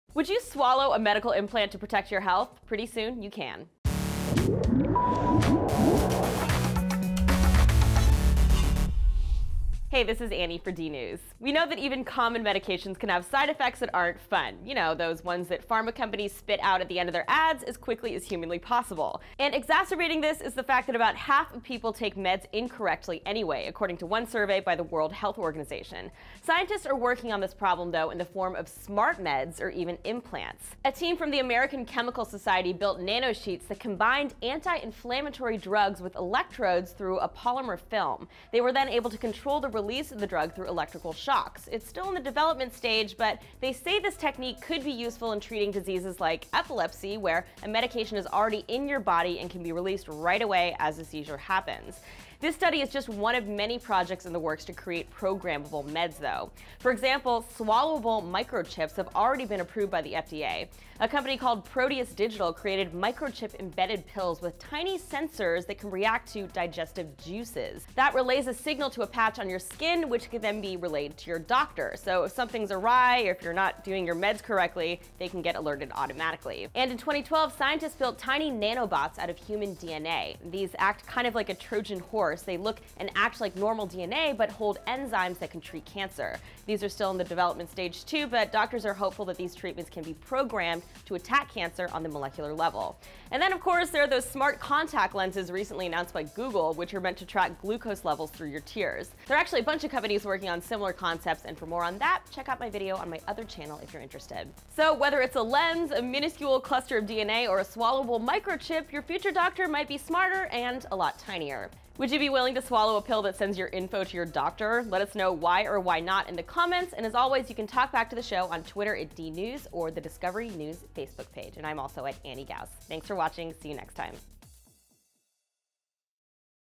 Digital Medicine Of The Future!
The fact is, more than half of all patients take their prescribed medications incorrectly. What if there was a way to ingest a microchip that would send a message to your doctor if you were taking a medicine wrong? Well, Annie is here to tell you about how this technology might be here sooner than you think.

Electrically Controlled Drug Delivery from Graphene Oxide Nanocomposite Films
"On-demand, local delivery of drug molecules to target tissues provides a means for effective drug dosing while reducing the adverse effects of systemic drug delivery."

"More than 50% of all medicines are prescribed, dispensed or sold inappropriately, and half of all patients fail to take medicines correctly."

"Digestible microchips embedded in drugs may soon tell doctors whether a patient is taking their medications as prescribed."

"Potential side effects of many of today's therapeutic drugs can be downright frightening - just listen carefully to a drug commercial on TV."

"DNA origami, a technique for making structures from DNA, may be more than just a cool design concept. It can also be used to build devices that can seek out and destroy living cells."

Temperature-Controlled Encapsulation and Release of an Active Enzyme in the Cavity of a Self-Assembled DNA Nanocage
"We demonstrate temperature-controlled encapsulation and release of the enzyme horseradish peroxidase using a preassembled and covalently closed three-dimensional DNA cage structure as a controllable encapsulation device."

"Someday you will take a pill that will fix your body. Sounds like science fiction and, though not yet science fact, it is a tantalizing potential presented by a breakthrough in nano technology and biomedicine from researchers in the U.S., Denmark and Rome."

"You've probably heard that diabetes is a huge and growing problem-affecting one in every 19 people on the planet. But you may not be familiar with the daily struggle that many people with diabetes face as they try to keep their blood sugar levels under control."